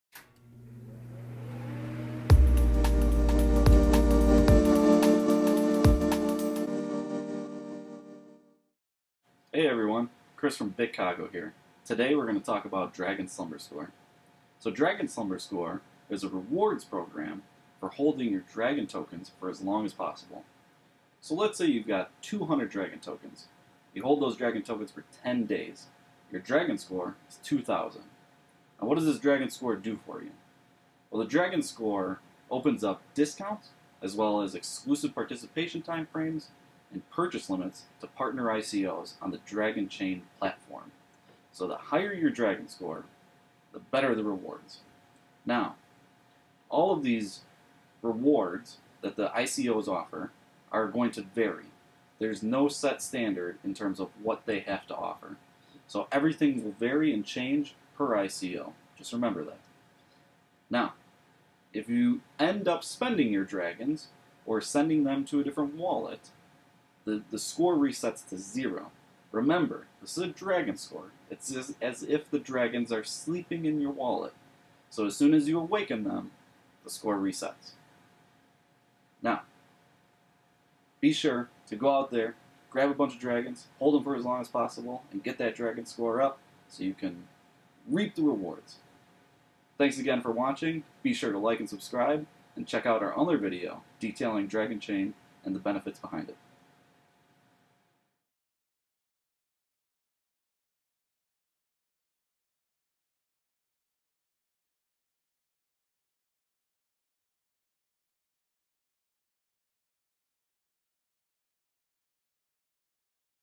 Dragon Slumber Score
Dragon slumber score is a rewards program for holding dragons for as long as possible. Dragons are the token used on the Dragonchain platform. The more dragons you hold in your wallet for as long as possible the greater the rewards for ICOs launching on the Dragonchain platform.